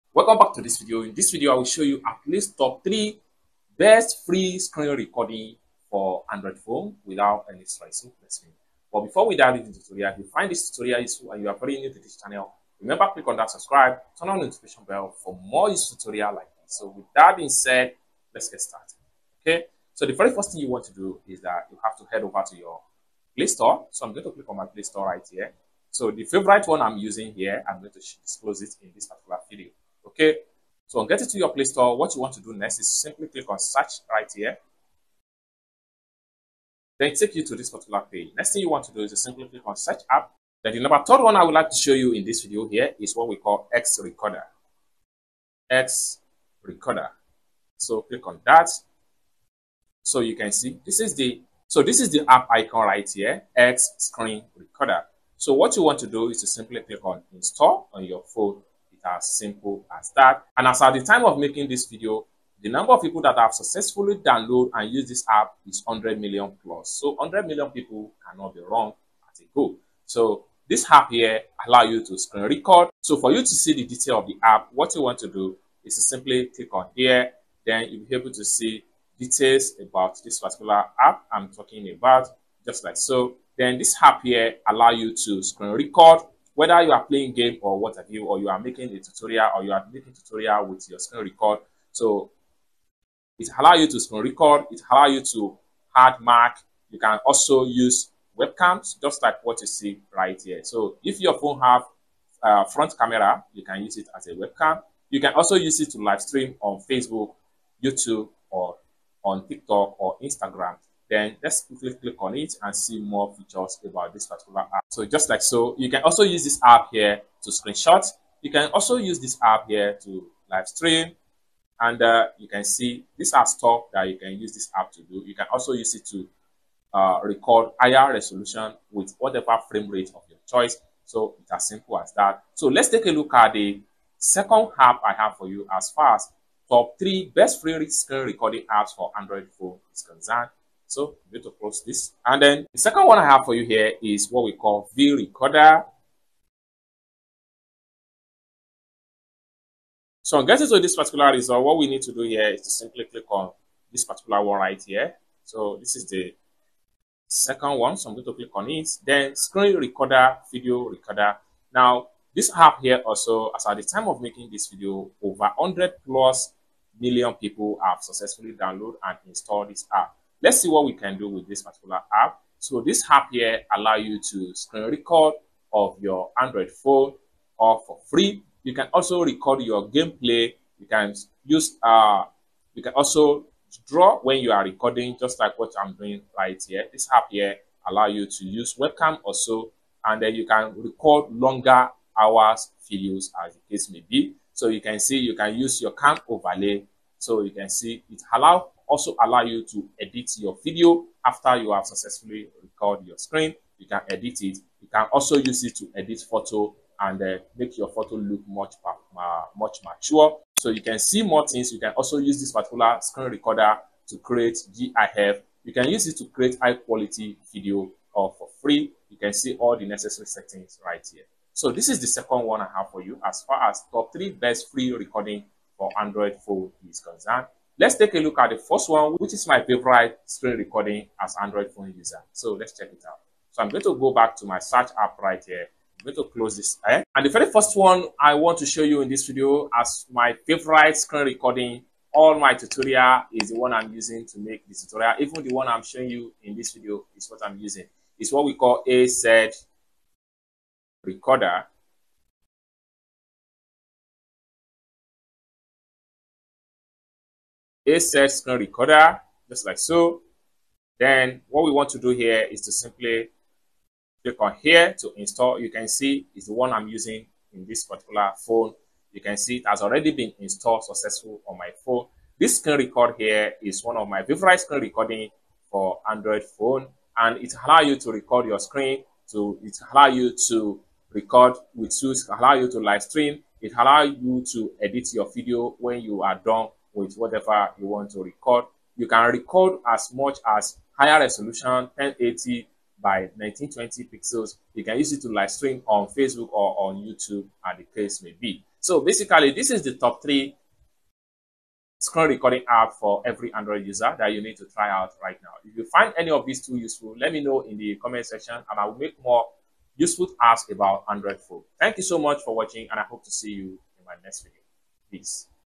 TOP 3 Best Free Screen Recording Apps - Android Device!
Are you looking for the best free screen recording apps for your Android device? Look no further! In this video, we dive into the top 3 screen recording apps that are not only free but also packed with amazing features to help you capture your screen effortlessly.

📈 Tips for Better Screen Recording:
- Ensure your device has enough storage space.
- Close unnecessary apps to improve performance.
- Use a stable surface or tripod to avoid shaky recordings.
- Test your settings before recording important content.

📢 Share Your Thoughts:
Have you tried any of these screen recording apps? Which one is your favorite?